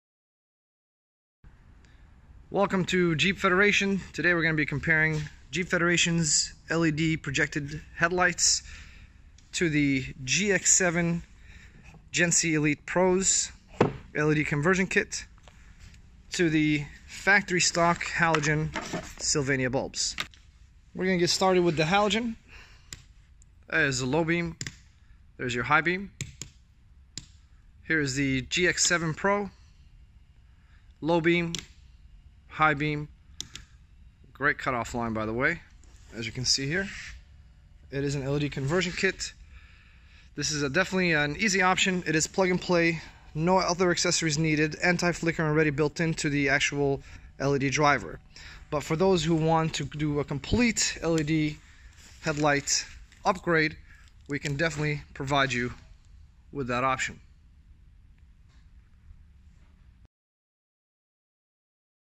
Jeep JL Headlight Comparison: Factory vs Halogen vs LED
Compare GX7 Pro LED headlight conversion kit with Halogen and CREE equipped projector headlamps.